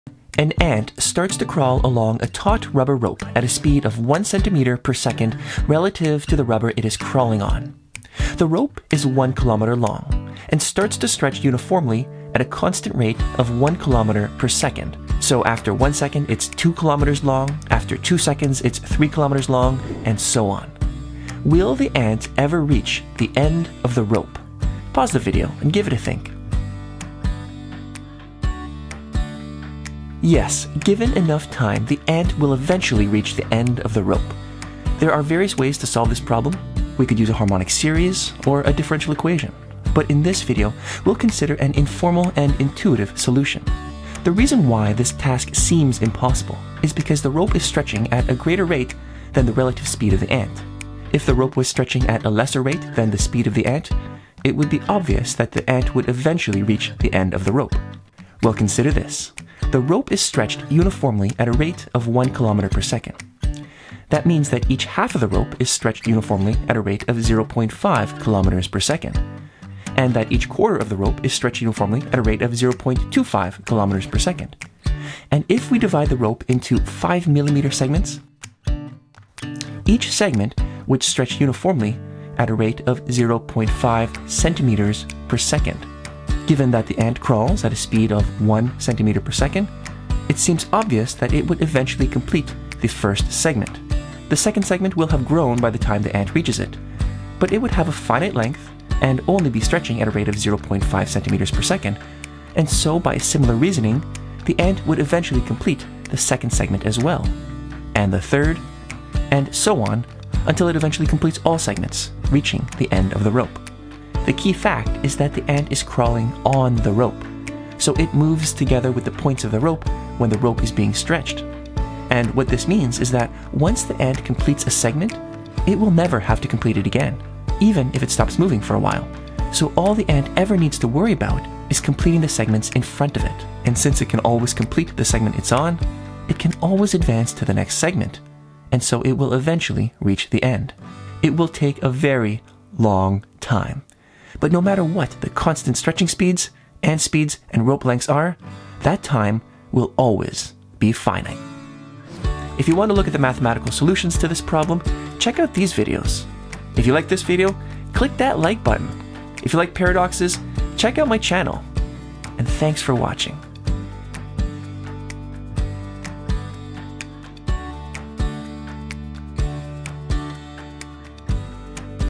An Intuitive Solution to the Ant on Rubber Rope Paradox
This video explores an informal and intuitive solution to the ant on a rubber rope paradox. The paradox is as follows: An ant starts to crawl along a taut rubber rope 1 km long at a speed of 1 cm per second (relative to the rubber it is crawling on). At the same time, the rope starts to stretch uniformly at a constant rate of 1 km per second, so that after 1 second it is 2 km long, after 2 seconds it is 3 km long, etc. Will the ant ever reach the end of the rope?